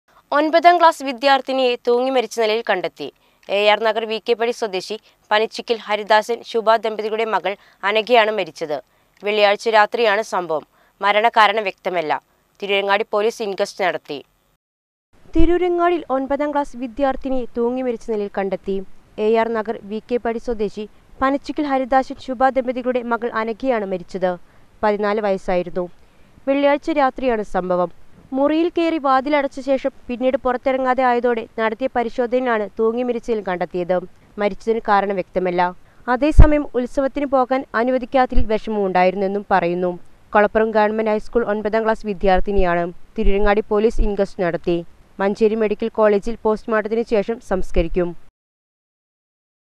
ഒന്‍പതാം ക്ലാസ് വിദ്യാര്‍ഥിനിയെ തൂങ്ങിമരിച്ച നിലയില്‍ കണ്ടെത്തി | Malayalam News
ഒന്‍പതാം ക്ലാസ് വിദ്യാര്‍ഥിനിയെ തൂങ്ങിമരിച്ച നിലയില്‍ കണ്ടെത്തി. എആര്‍ നഗര്‍ വി കെ പടി സ്വദേശി പനച്ചിക്കല്‍ ഹരിദാസന്‍  ശുഭ ദമ്പതികളുടെ  മകള്‍ അനഘയാണ് മരിച്ചത്.വെള്ളിയാഴ്ച രാത്രിയാണ് സംഭവം.